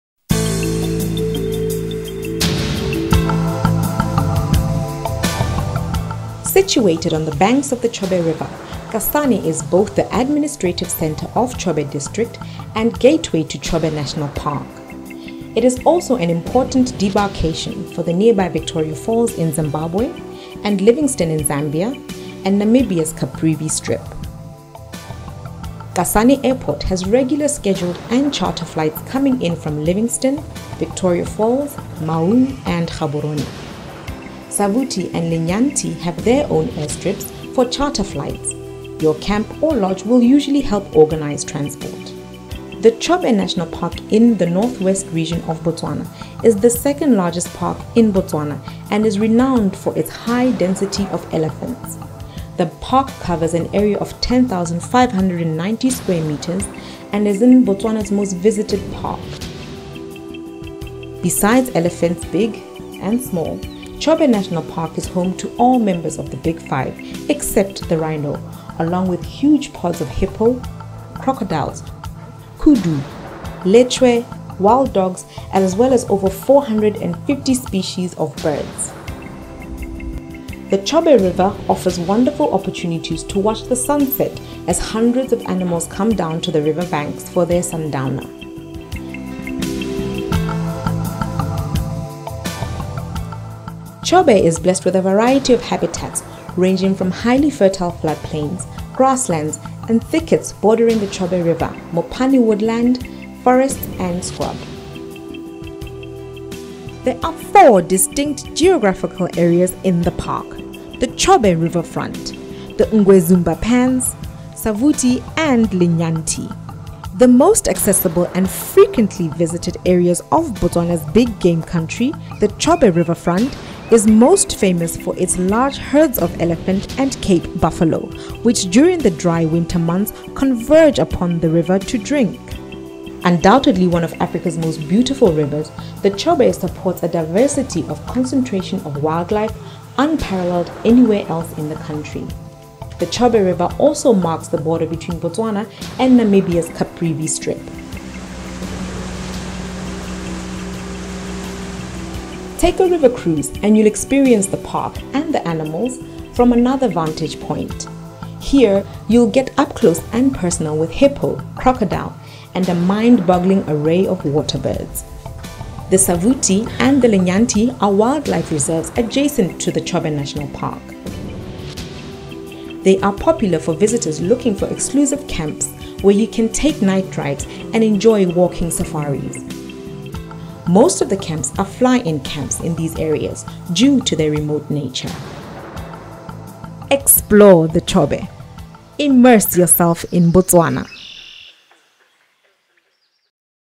Welcome to Botswana: Chobe
Situated on the banks of the Chobe River, Kasane is both the administrative center of Chobe District and gateway to Chobe National Park.